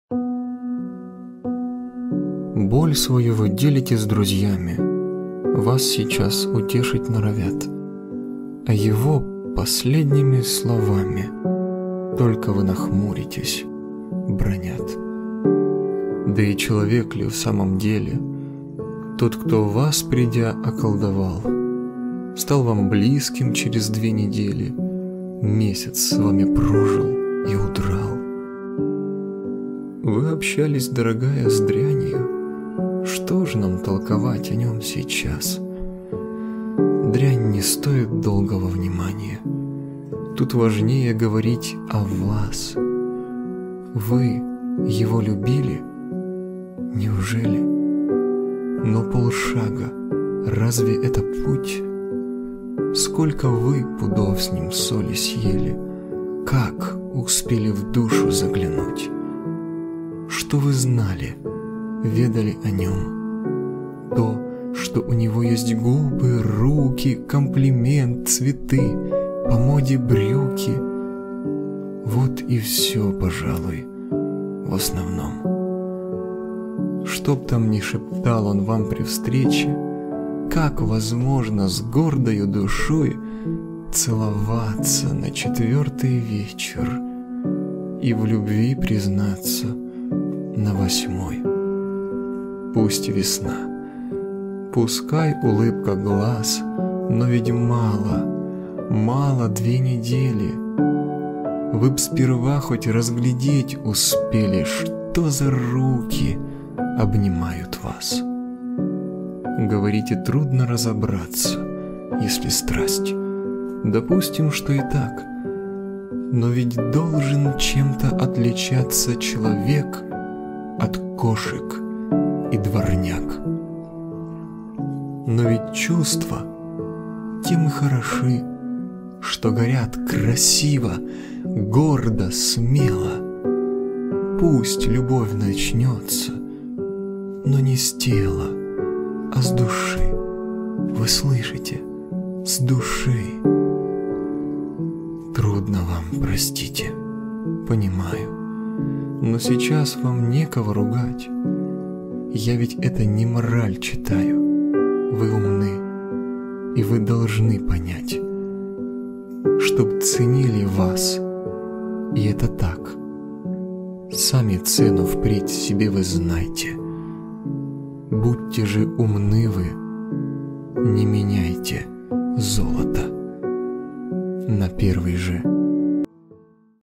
вы его любили? неужели?... Э.Асадов
Стихи о любви... Прямой разговор - Э. Асадов

Читает: Анатолий Супрунюк
Автор: Эдуард Асадов

Текст стихотворения:

Боль свою вы делите с друзьями,
Вас сейчас утешить норовят,
А его последними словами,
Только вы нахмуритесь, бранят.

Да и человек ли, в самом деле,
Тот, кто вас, придя, околдовал,
Стал вам близким через две недели,
Месяц с вами прожил и удрал?

Вы общались, дорогая, с дрянью.
Что ж нам толковать о нем сейчас?!
Дрянь не стоит долгого вниманья,
Тут важнее говорить о вас.

Вы его любили? Неужели?
Но полшага — разве это путь?!
Сколько вы пудов с ним соли съели?
Как успели в душу заглянуть?!

Что вы знали, ведали о нем?
To, что у него есть губы, руки,
Комплимент, цветы, по моде брюки —
Вот и все, пожалуй, в основном?

Что б там ни шептал он вам при встрече,
Как возможно с гордою душой
Целоваться на четвертый вечер
И в любви признаться на восьмой?!

Пусть весна, пускай улыбка глаз…
Но ведь мало, мало две недели!
Вы б сперва хоть разглядеть успели,
Что за руки обнимают вас!

Говорите, трудно разобраться,
Если страсть. Допустим, что и так.
Но ведь должен чем-то отличаться
Человек от кошек и дворняг!

Но ведь чувства тем и хороши,
Что горят красиво, гордо, смело.
Пусть любовь начнётся. Но не с тела,
А с души, вы слышите, — с души!

Трудно вам. Простите. Понимаю.
Но сейчас вам некого ругать.
Я ведь это не мораль читаю,
Вы умны, и вы должны понять:

Чтоб ценили вас, и это так,
Сами цену впредь себе вы знайте.
Будьте горделивы. Не меняйте
Золота на первый же медяк!